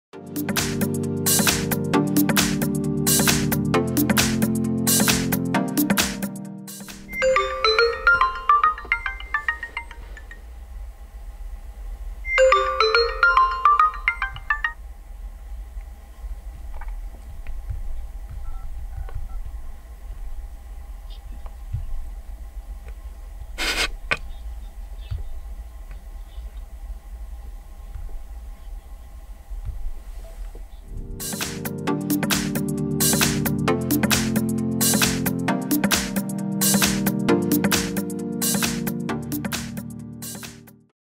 Setting up an incoming call on an HTC Desire 22 Pro phone
Today we will look at what functions are available in the phone during an incoming call. Our employee will show you what certain functions in the phone are responsible for when calling. Follow his instructions and you will know a little more.

What does the incoming call menu look like? How do I set up my phone correctly when receiving an incoming call? What functions exist when making a call? What is interesting in the call menu in the htc desire 22 pro phone?